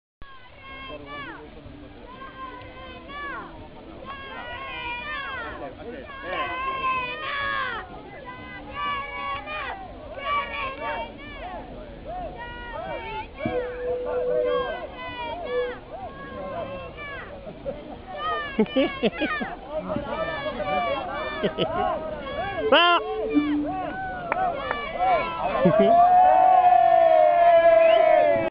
larrabetzuko jaiak 06
LA CUADRILLA  ONENEAK
EN ACCION EN LA BAJADA DE GOITIBEHERAS